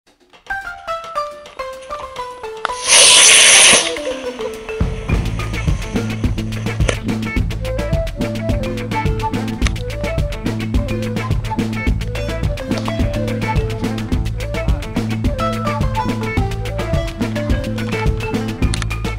mini rocket attached to toy car fail
I attached an Estes rocket from Toys R Us to a Nascar toy model with tape and tried to get a takeoff launch. This was the result. I guess I’m not going to NASA.